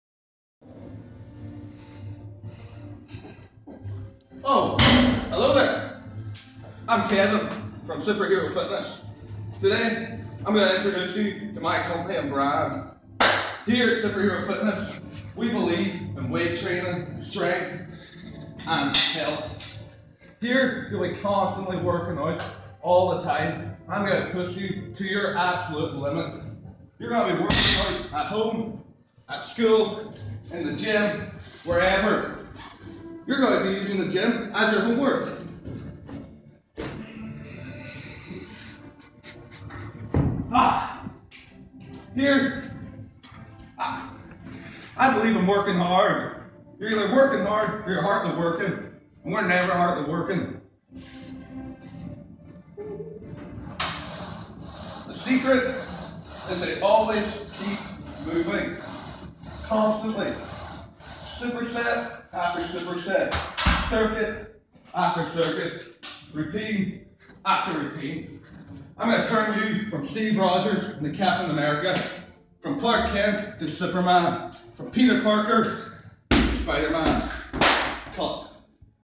superhero fitness!!!! (piss take)- how to succeed in the gym!
This was a video I made for my sport and exercise science course, I was told to make an introduction video for my "personal training business" and instead made this. Enjoy and have a good laugh at my autistic af ass(also size of my arms)